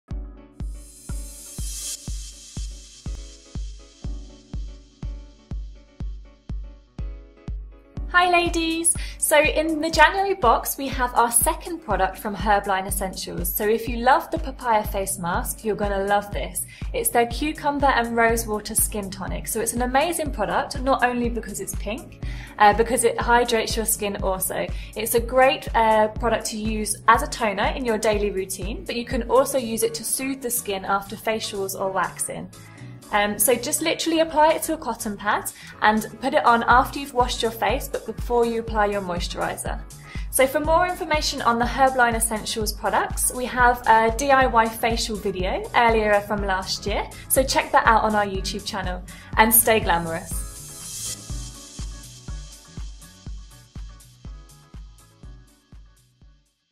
The January 2013 GlamBox - Herbline Essentials - Rosewater and Cucumber Toner
The January GlamBox - Herbline Essentials - Rosewater and Cucumber Toner

One of our favourite natural brands Herbline Essentials have given us this amazing Rosewater and Cucumber toner - the colour pink is perfect for Valentines day which is around the corner!
Contains pure natural herb extracts and cucumber and rose, to cool and soften the skin. Blended with a natural moisturiser and a mild astringent, it soothes and tones leaving supple, moist and fresh looking skin. The toner is also recommended for use after facials, shaving, removal of blackheads and waxing.

GlamBox.ME - Experience the latest skincare, haircare, fragrances and makeup from the hottest brands in the comfort of your own home.